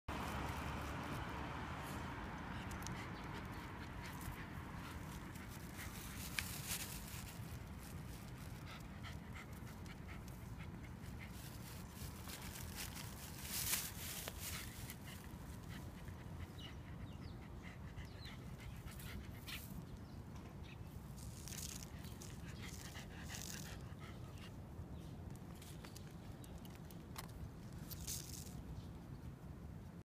柴犬 スペックの散歩 尼崎築地
尼崎 築地公園です。
朝から良い天気です。
20150610